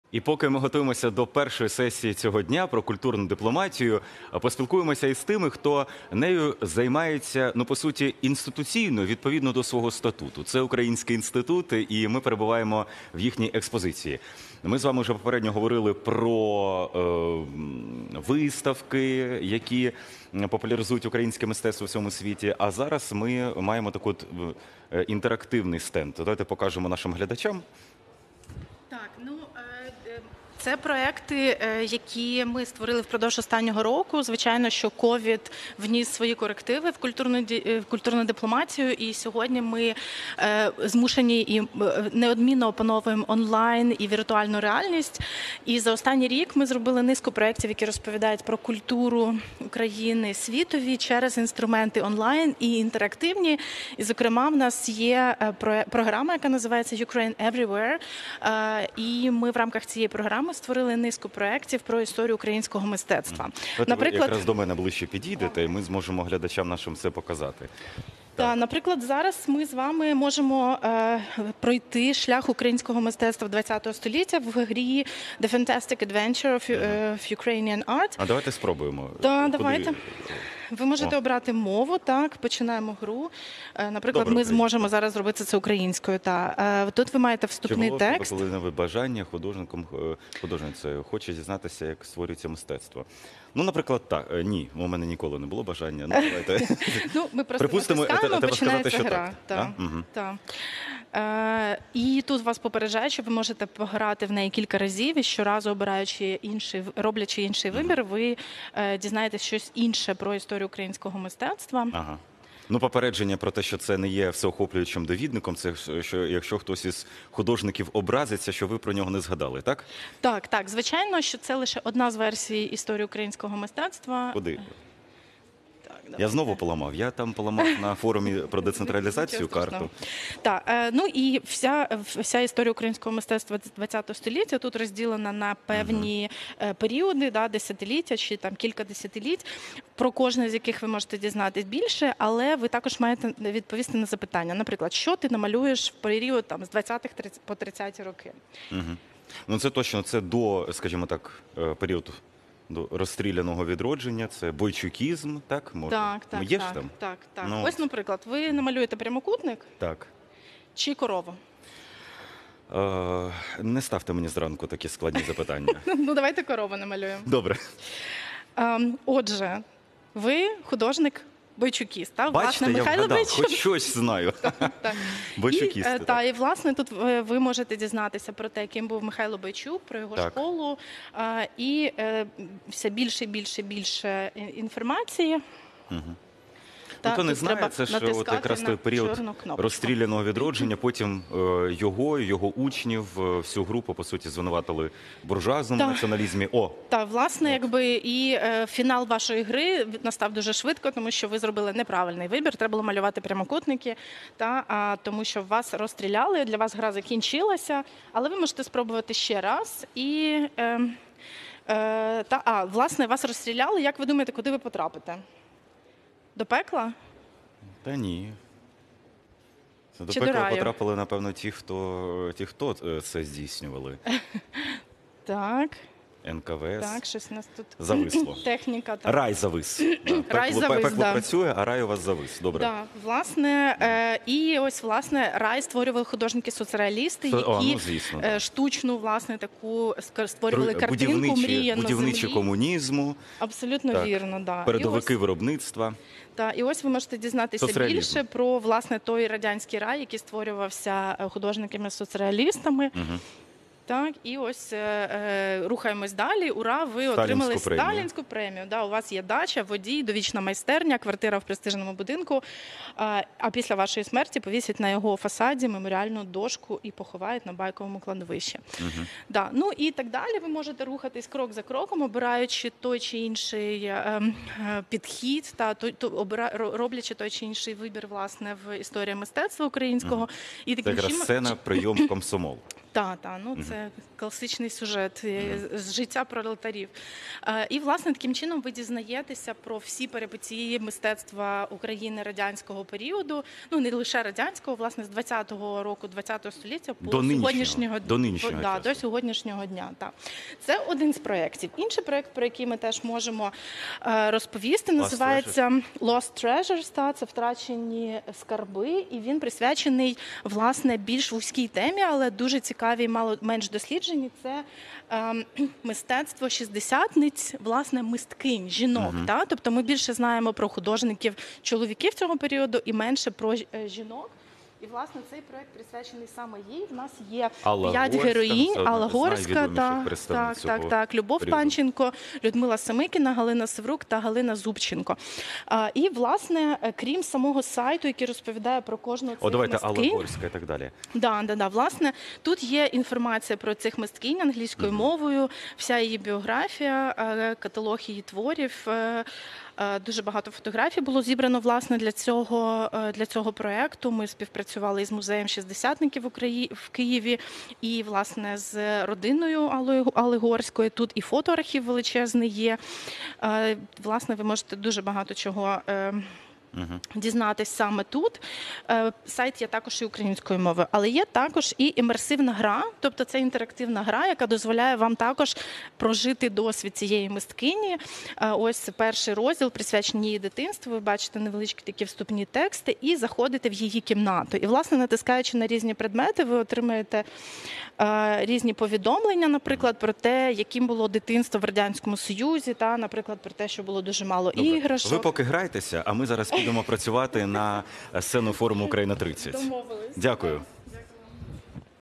Експозона на Всеукраїнському Форумі «Україна 30. Імідж України»
На Всеукраїнському Форумі «Україна 30. Імідж України» була представлена експозона з культурними проєктами Українського інституту, голове завдання яких - розповісти світові про Україну. Подробиці - у нашому відео.